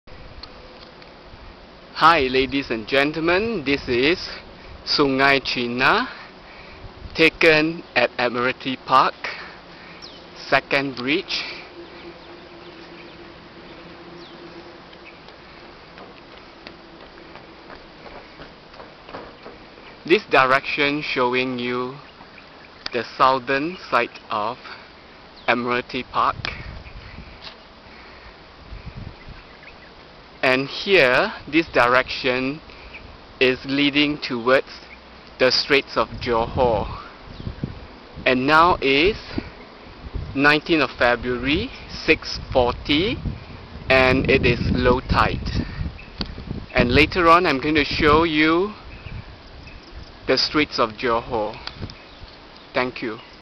Admiralty Park, Sungei Cina Part 1 (19 Feb 2009)
This river runs from Admiralty Park South all the way to the Straits of Johor. This short video is an attempt to trace the sources of the this unique river, i.e. one end with freshwater origin and the other with seawater.